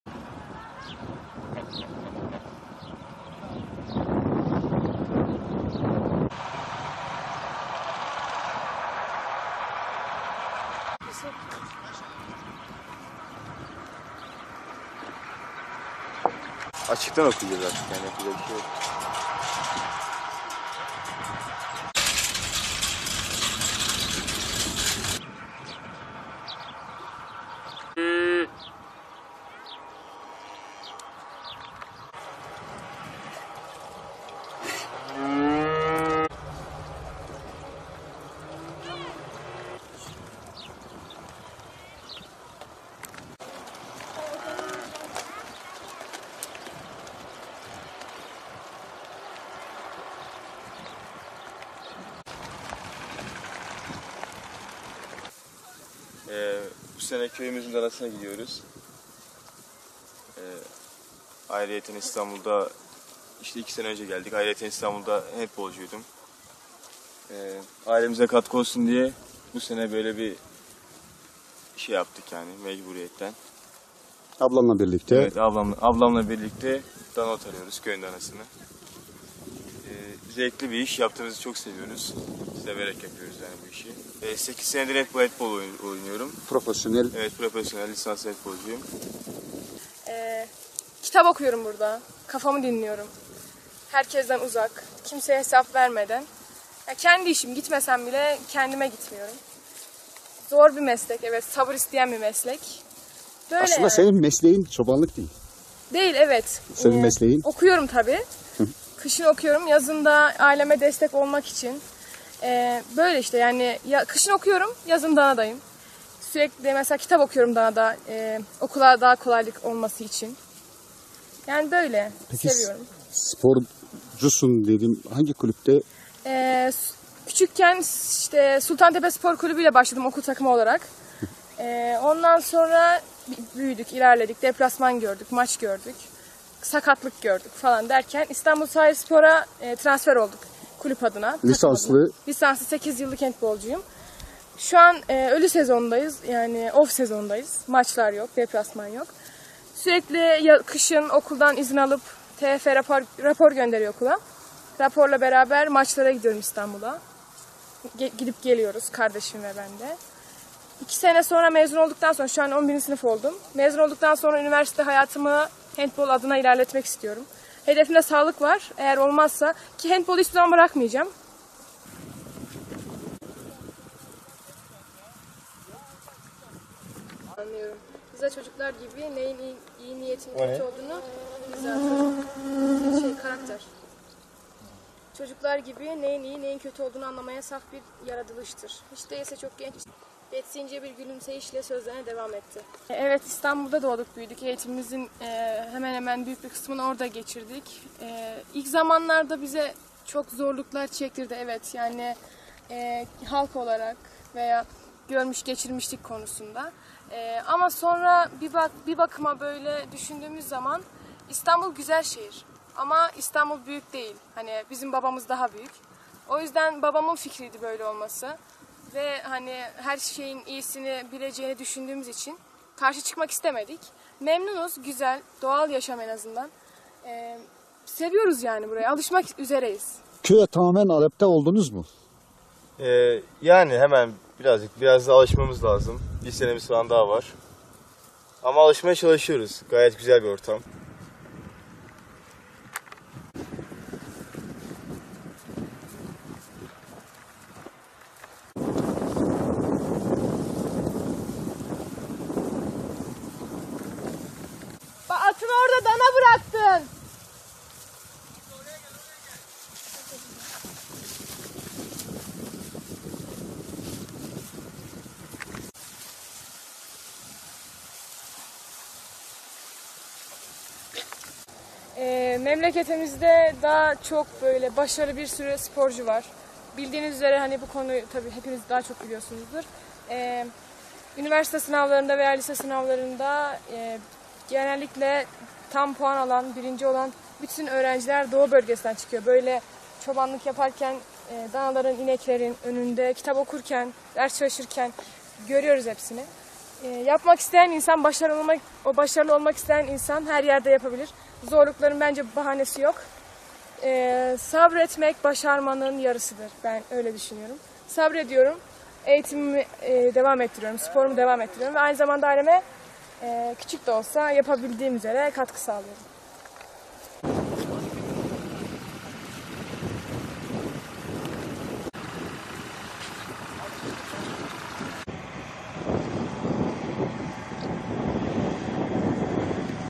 Hentblcu kardeşler köylerinde çobanlık yapıyor
DHA muhabiri Suat İncedere, Hentbolcu iki kardeş Kübra ve Tamer Sarıkaya ile yapmış olduğu özel haber.

İstanbul'da farklı kulüplerde hentbol oynayan Kübra (17) ve Tamer Sarıkaya (16) kardeşler, ailelerine maddi destek sağlamak için memleketleri Ardahan'da çobanlık yapıyor.